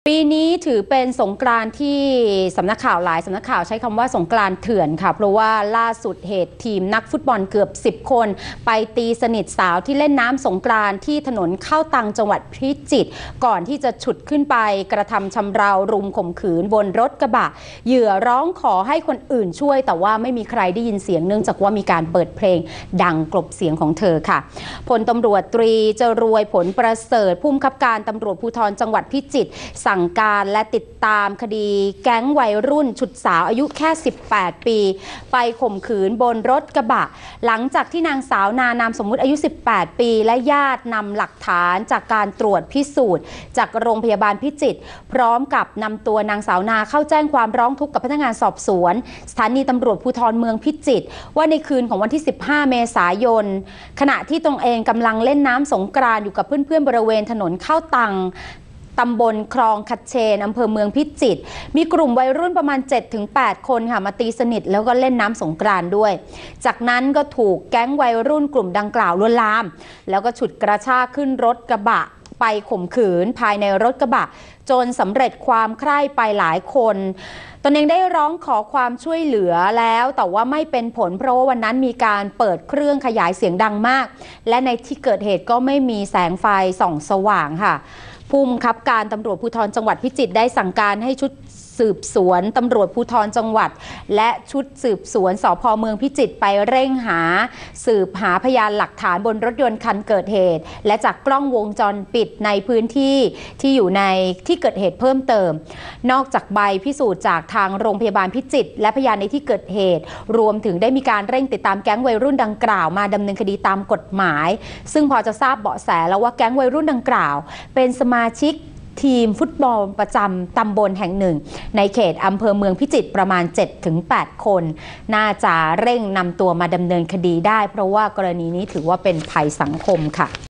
สาว 18 หอบผลตรวจแพทย์แจ้ง ตร.ล่าแก๊งโจ๋ฉุดเวียนข่มขืนคารถคืนสงกรานต์
23/04/2017 สาว 18 หอบผลตรวจแพทย์แจ้ง ตร.ล่าแก๊งโจ๋ฉุดเวียนข่มขืนคารถคืนสงกรานต์